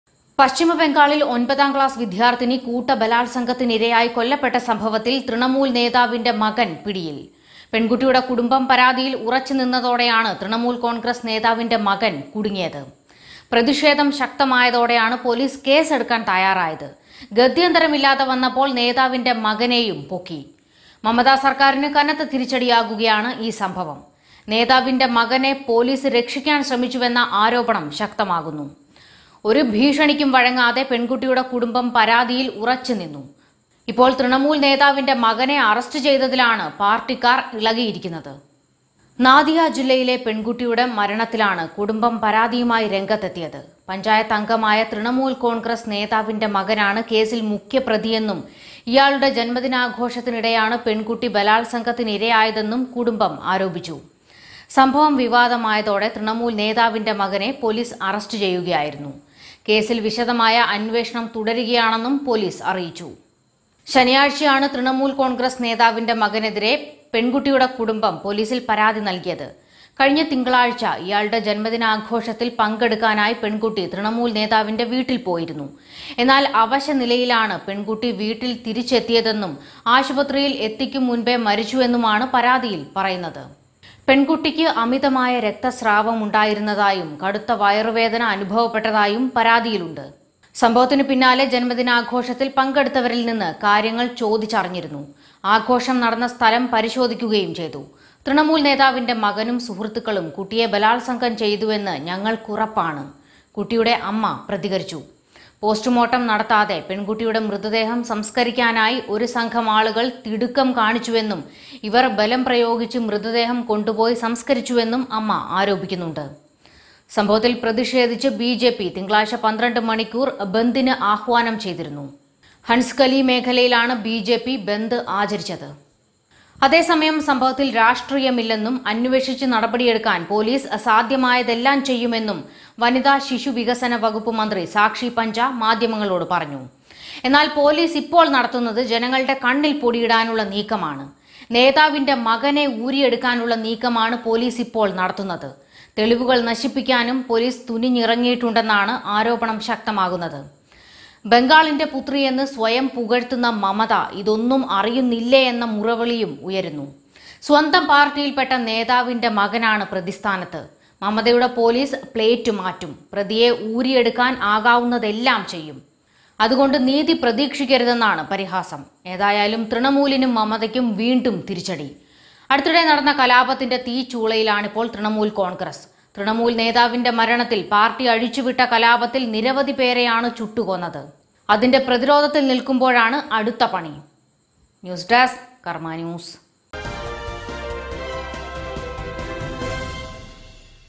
പെണ്‍കുട്ടിയെ നശിപ്പിച്ചു;തൃണമൂല്‍ നേതാവിന്റെ മകന്‍ കുടുങ്ങി
പ്രായപൂര്‍ത്തിയാകാത്ത പെണ്‍കുട്ടി മരിച്ച സംഭവം,ബംഗാളില്‍ തൃണമൂല്‍ നേതാവിന്റെ മകന്‍ അറസ്റ്റില്‍